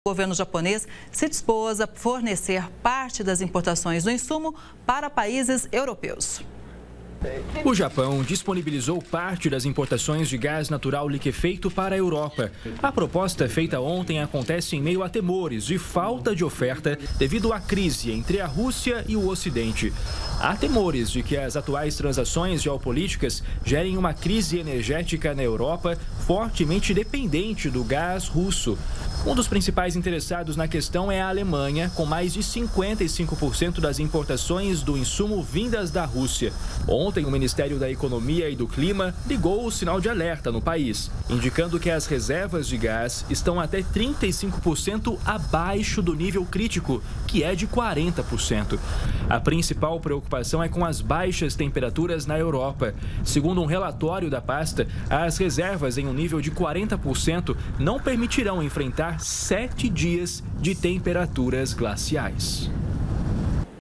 Japão oferece fornecimento de gás à Europa
Com toda a tensão com a Rússia, o fornecimento de gás para a Europa é uma das principais preocupações. Durante a semana, o governo japonês se dispôs a fornecer parte das importações do insumo para países europeus.